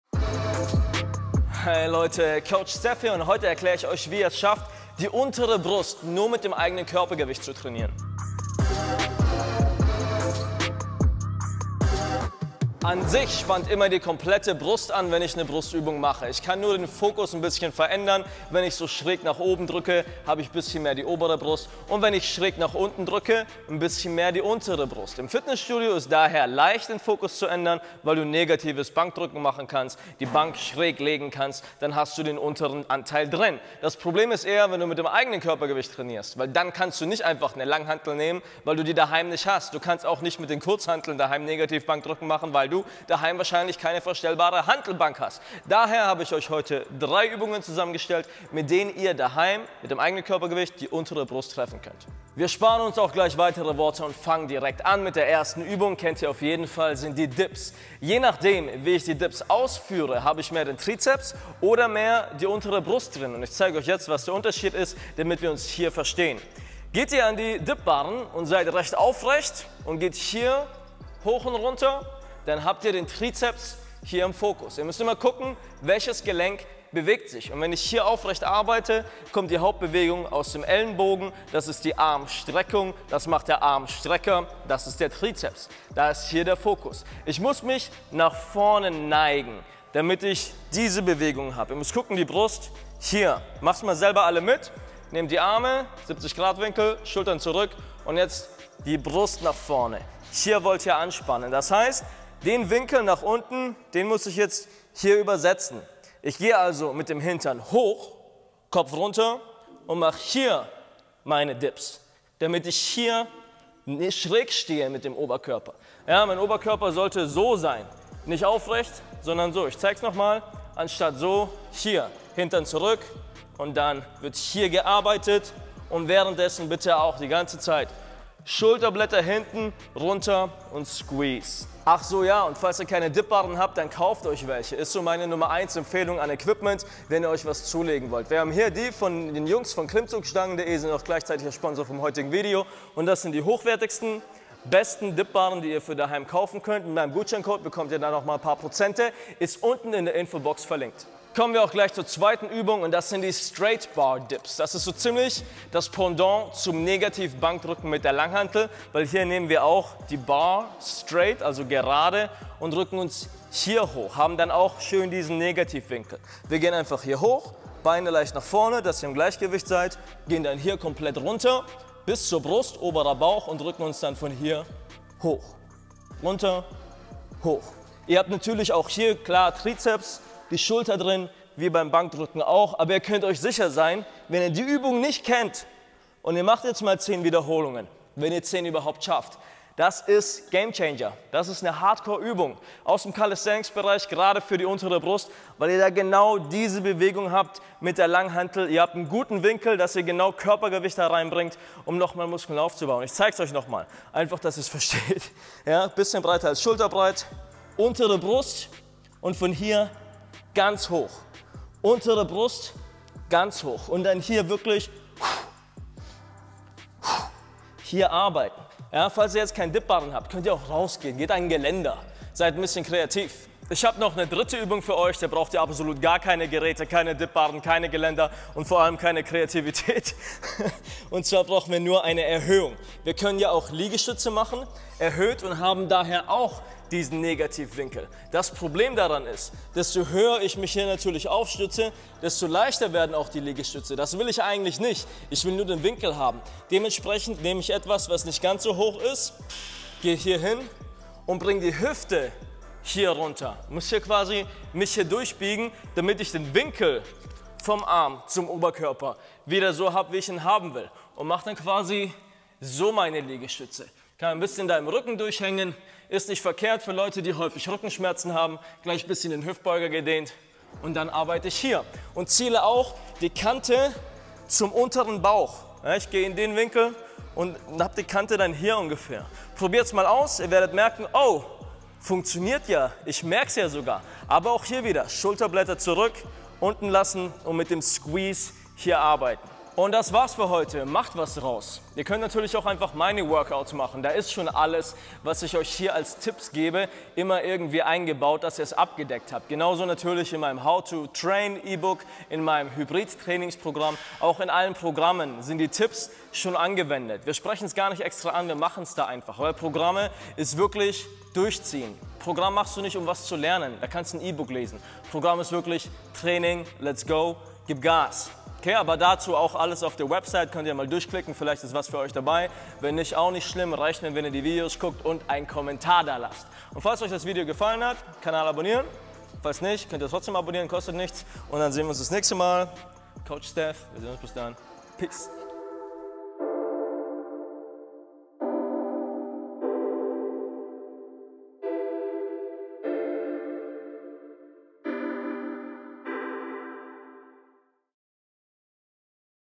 UNTERE BRUST mit dem eigenen Körpergewicht trainieren (Bodyweight Übungen)
Heute im Video zeige ich euch wie ihr die UNTERE BRUST mit dem eigenen Körpergewicht trainieren könnt. Dazu habe ich 3 Bodyweight Übungen ausgesucht, die ihr zuhause nachmachen könnt. Baut die Übungen einfach in euer Training ein oder macht gleich meine Workouts. Welche Körperpartie soll ich als nächstes erklären? Schreibt es in die Kommentare!

Dip Barren

Mein Pre-Workout von NOCCO:
ZUM SHOP